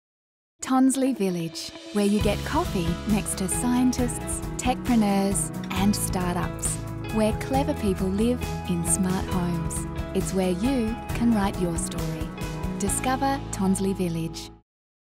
Tonsley Village - The Smartest
Tonsley Village - It’s where you’ll find thought leaders, global brands, new ways of thinking, making, learning and doing; making it one of the most desirable places to live.

Find out more and register your interest today at tonsleyvillage.com.au